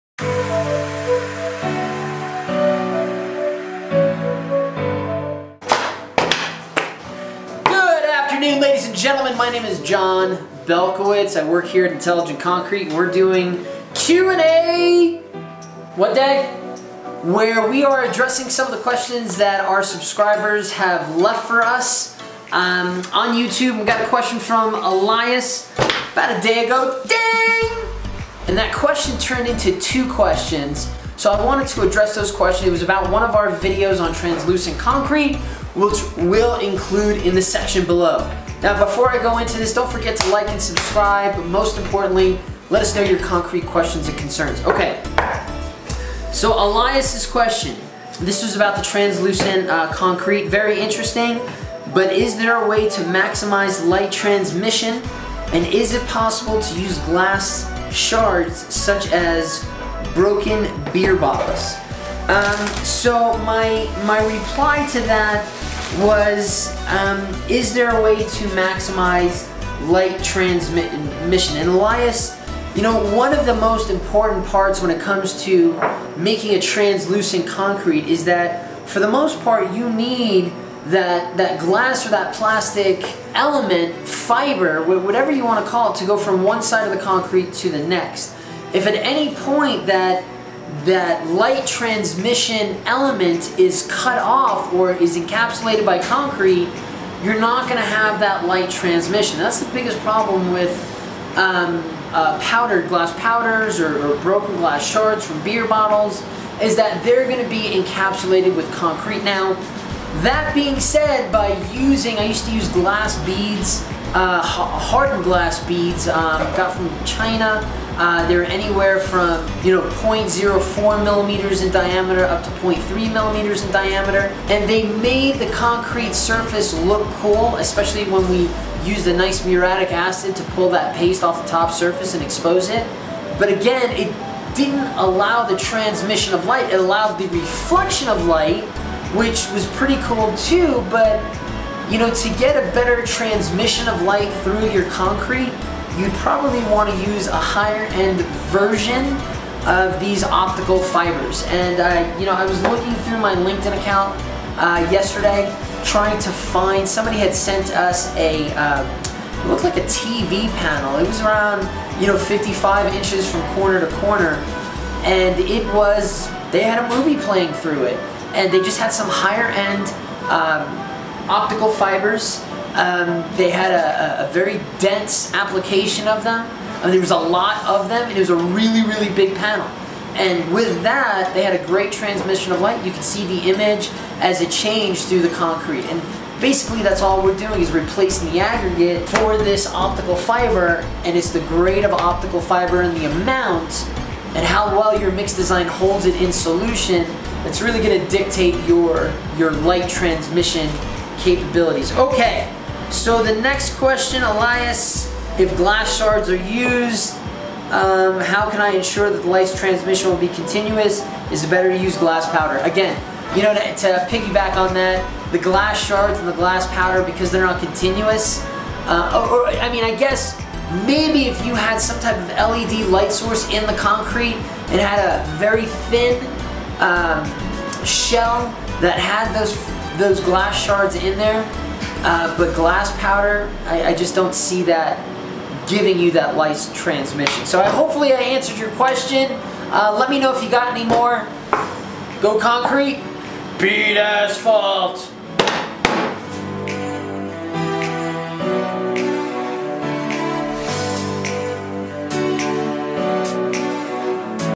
Can We Use Glass For Translucent Concrete? - Vlog #145
Join Jon & the Intelligent Concrete team in their concrete testing lab to learn the answer to the question: Can We Use Glass For Translucent Concrete?

CONCRETE MERCH!